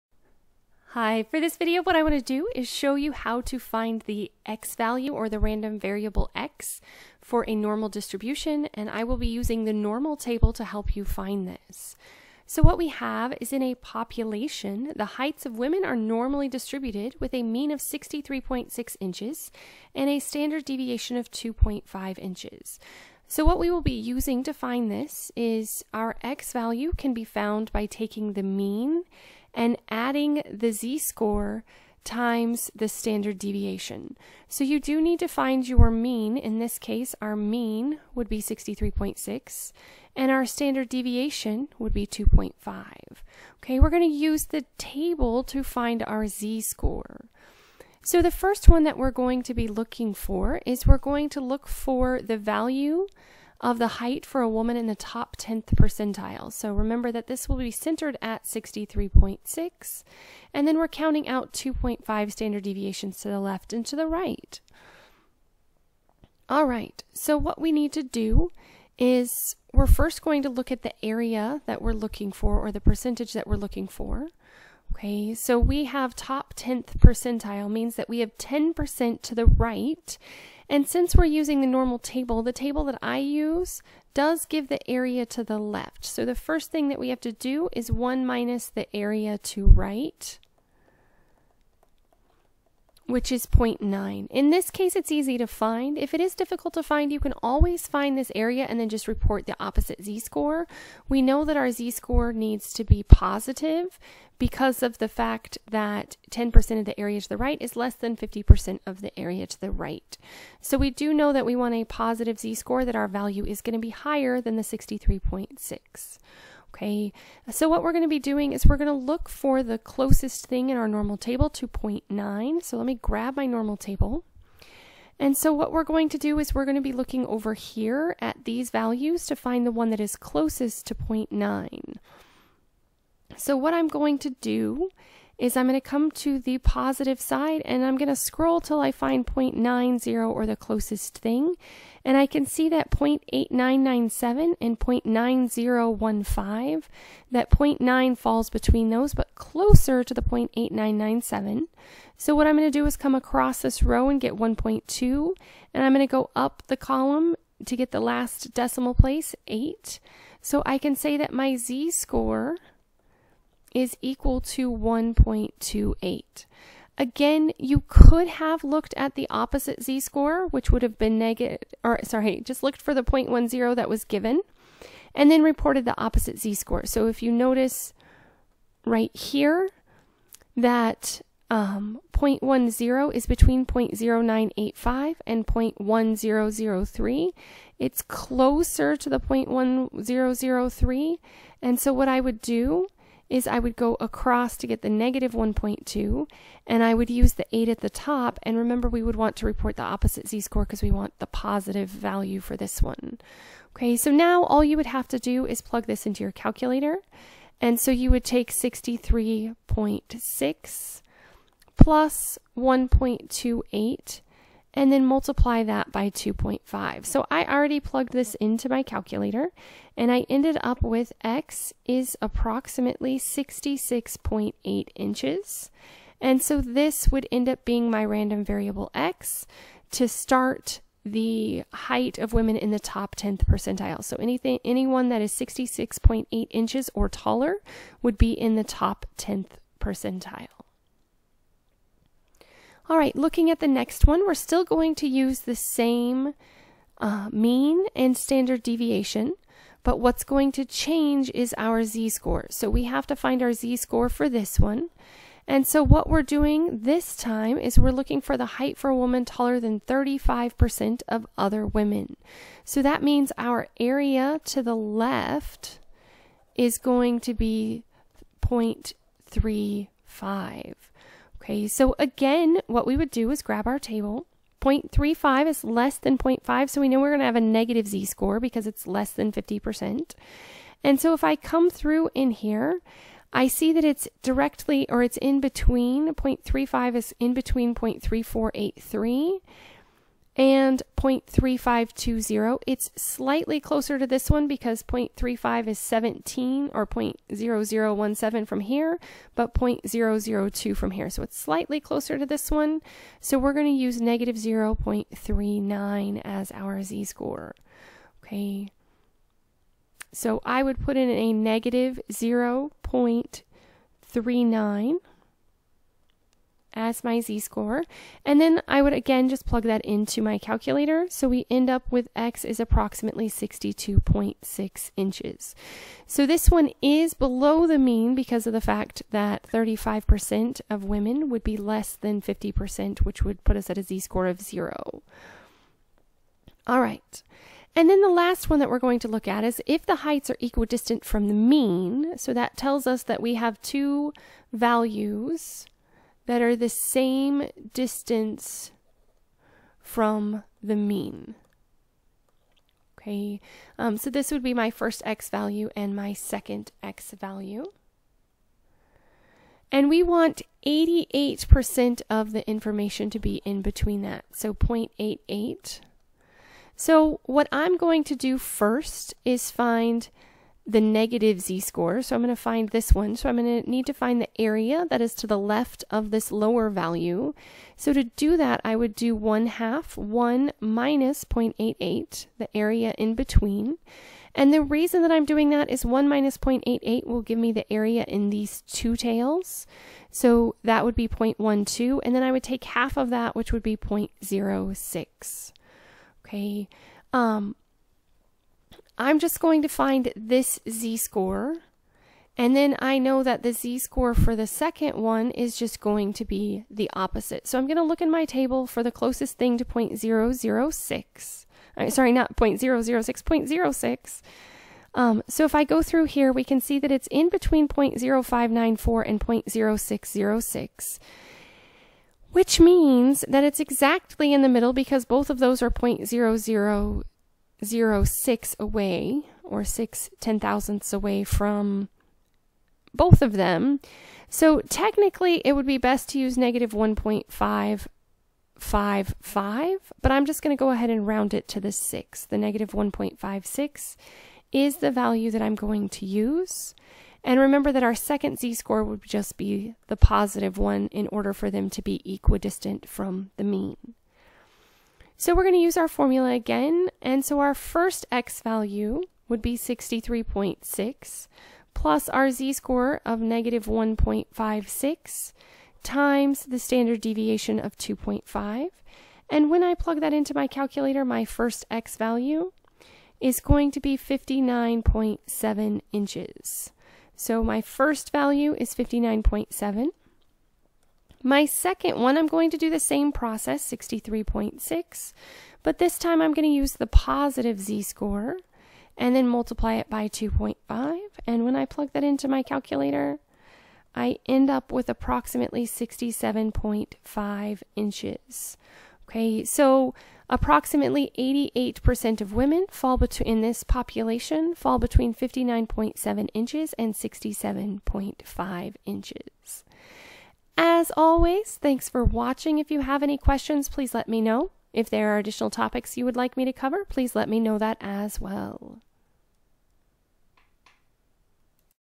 Find X-value for Normal Distribution Using the Normal Table
This video demonstrates how to find the x-value or random variable x using the normal table in a normally distributed population. If you are using the table, you must first find the z-score that corresponds to the given percentage or area of the normal curve, then convert the z-score into the x-value.